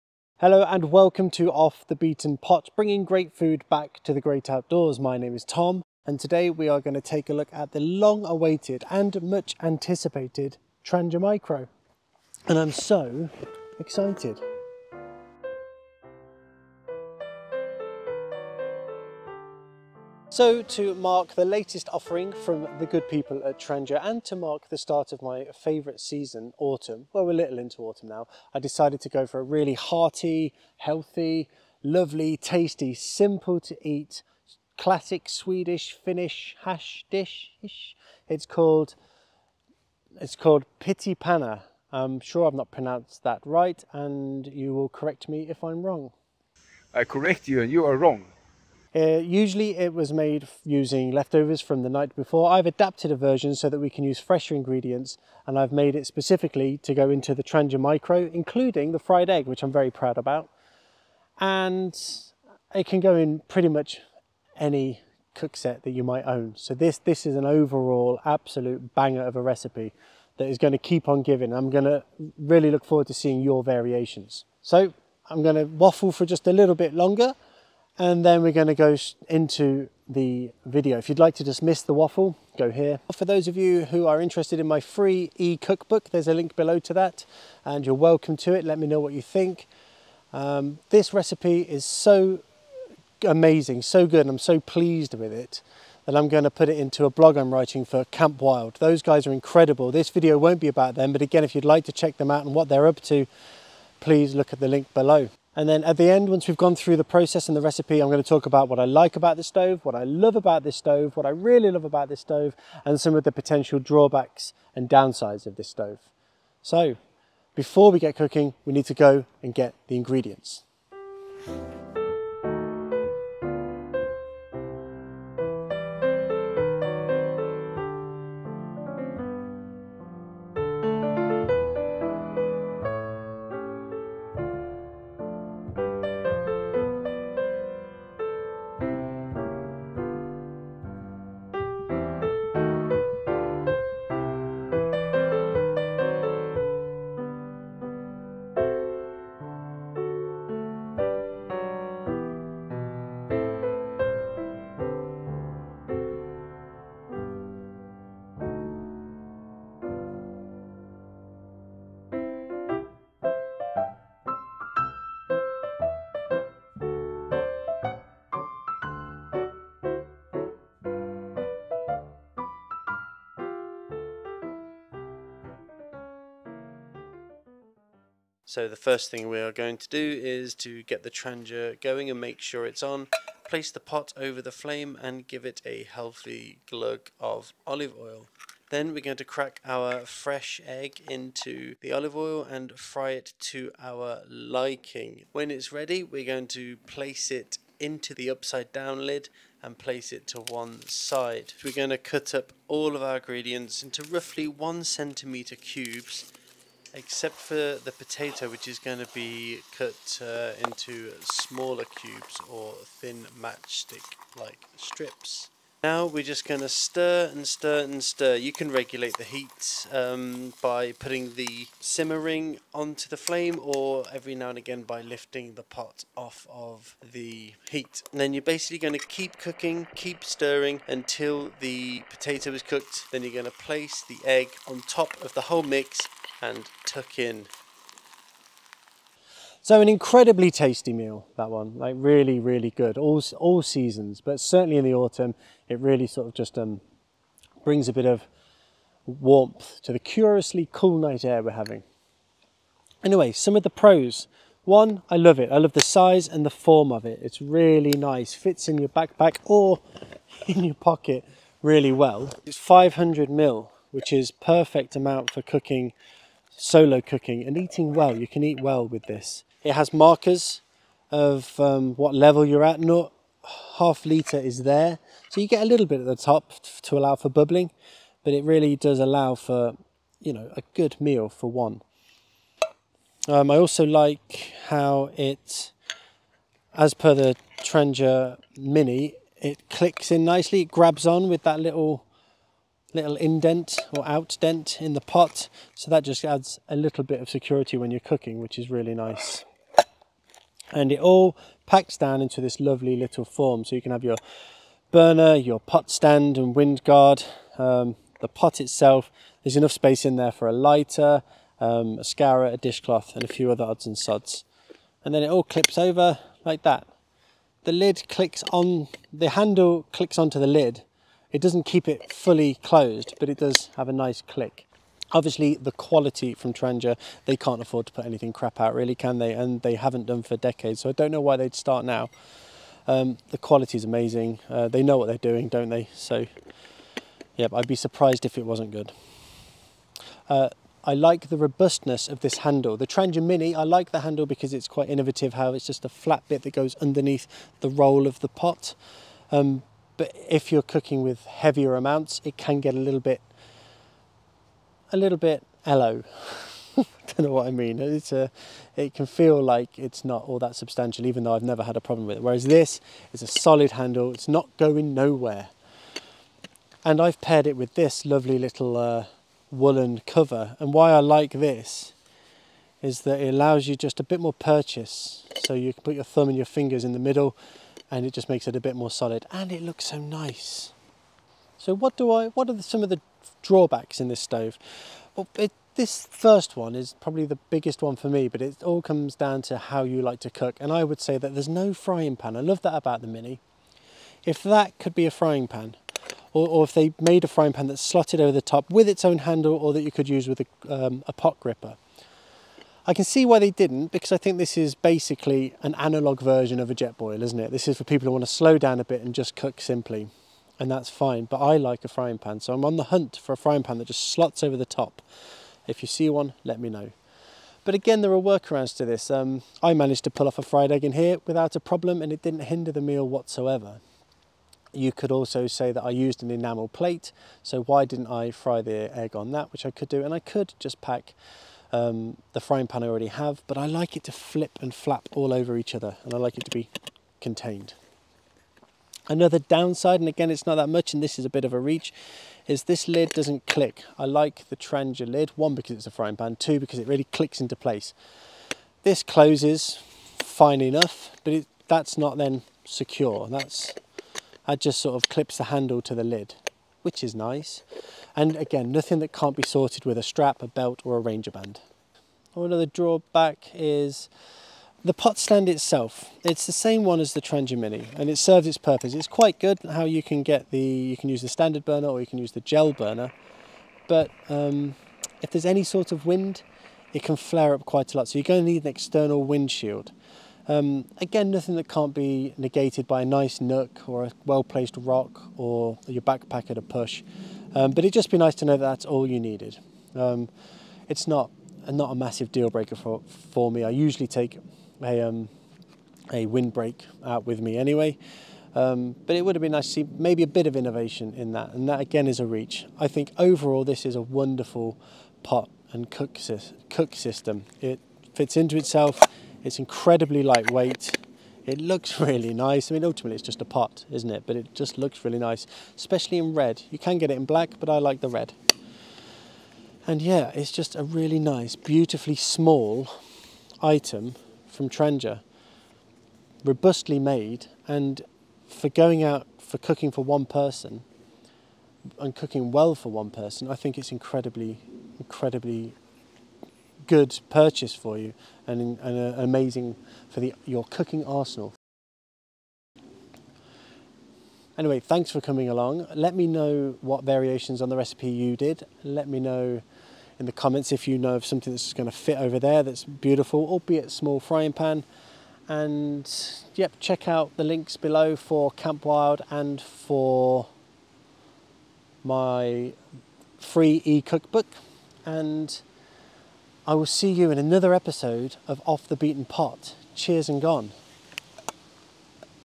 Trangia Micro REVIEW! Is it worth it? Cooking a traditional Swedish meal to find out.
The Trangia Micro is the much anticipated new release from Trangia. I have had mine for a couple of weeks now and I've been putting it to the test, cooking up tasty hiking meals while in the great outdoors!
What better way to test this new camping stove than cooking up a traditional Swedish Meal.

🧑‍🍳My Ultimate Cooking Utensil